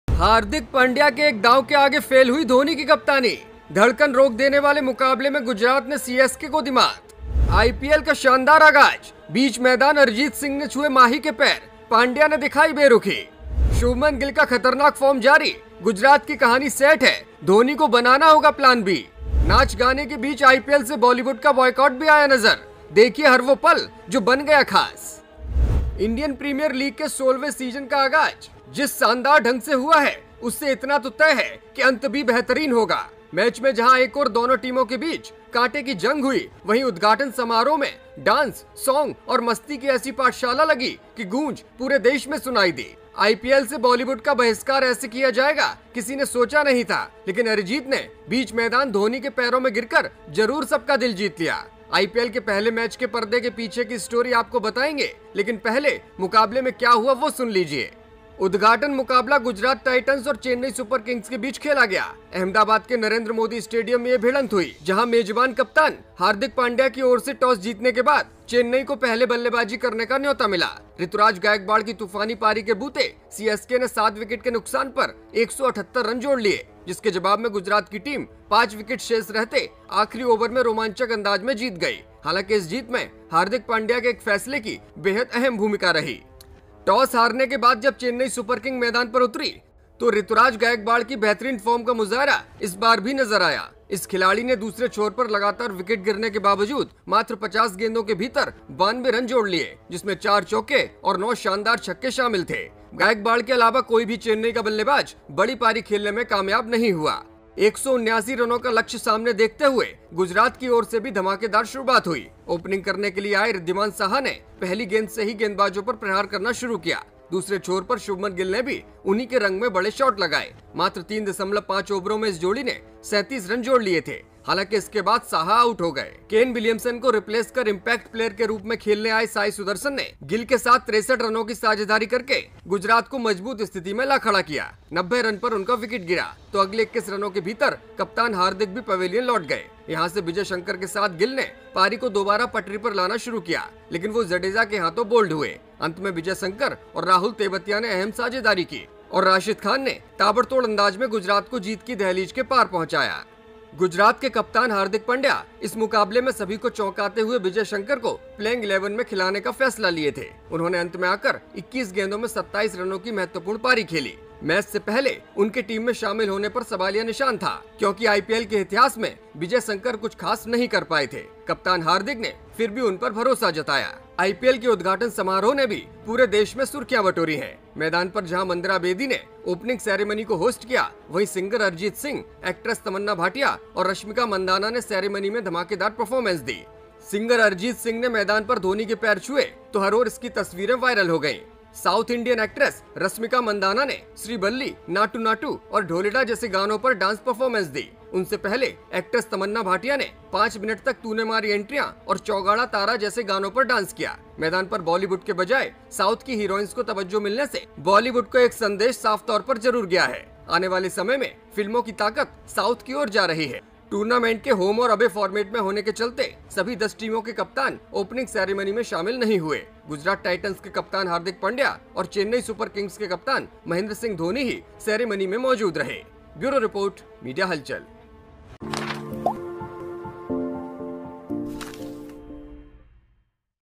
हार्दिक पंड्या के एक दांव के आगे फेल हुई धोनी की कप्तानी! गिल का जलवा कायम, पैरों में गिर पड़े अरिजीत
Join Media Halchal to get access to perks:
हार्दिक पंड्या के एक दांव के आगे फेल हुई धोनी की कप्तानी! शुभमन गिल का जलवा कायम, पैरों में गिर पड़े अरिजीत